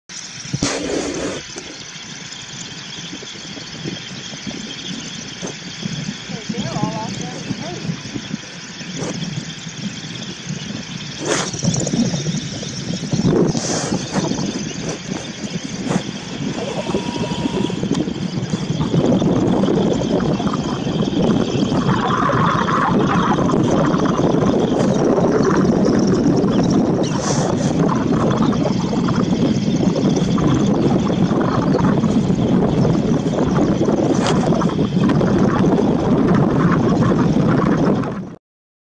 Semipalmated and White-rumped Sandpipers on James Bay
Flocks of shorebirds at high tide on 2 August 2015. James Bay and the Bay of Fundy are the two most important stopover sites for
southbound Semipalmated Sandpipers in North America. James Bay is the most important fall staging area for White-rumped Sandpiper in North America.